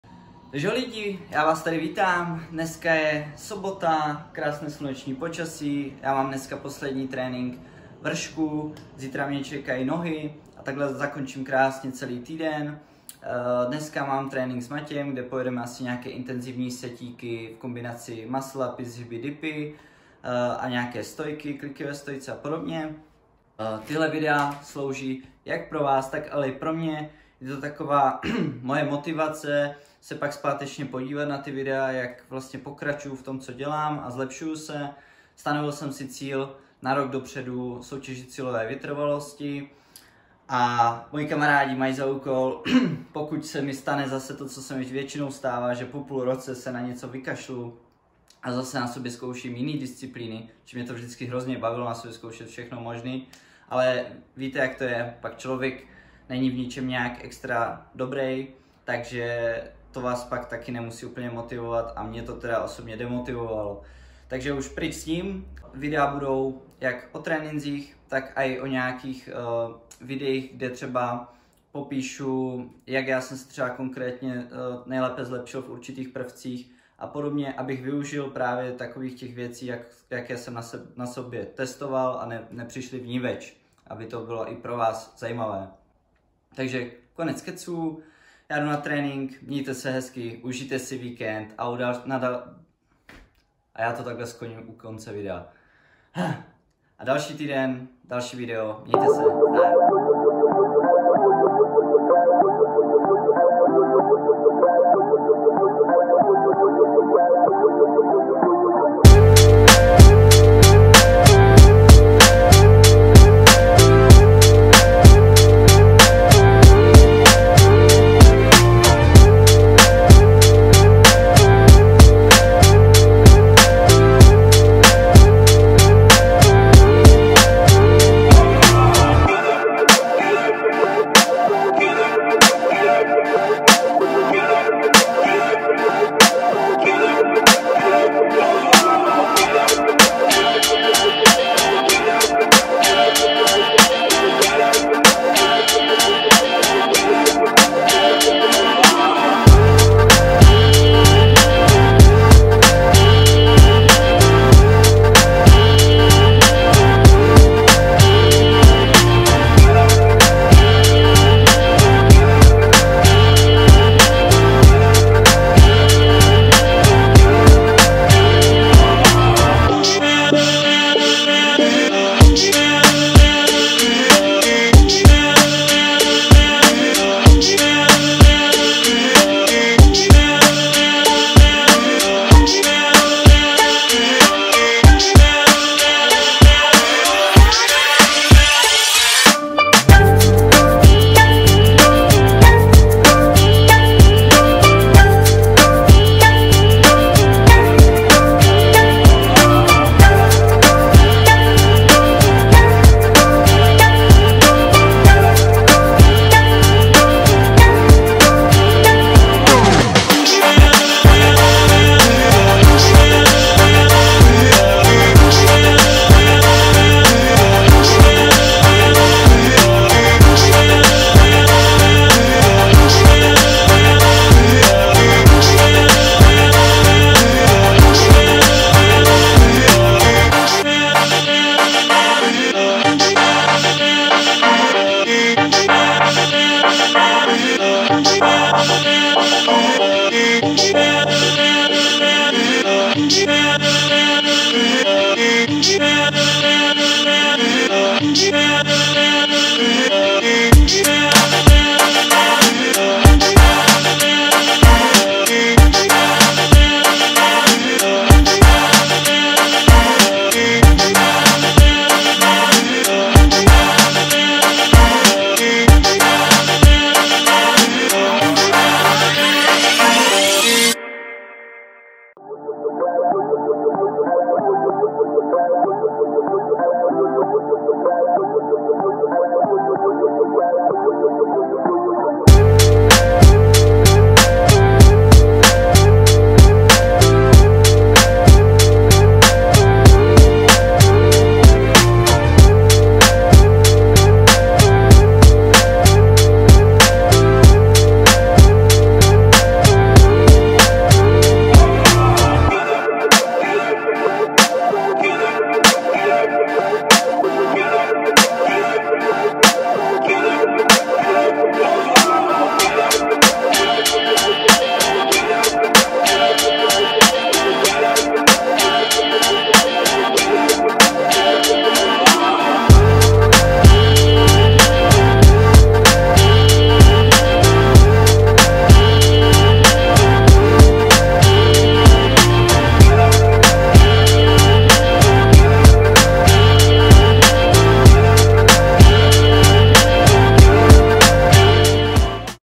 Trénink s tímto monstrem na úrovni!
Sobotní trénink s mým kamarádem jsme si užili na maximum. Je potřeba využít těch posledních teplých dní.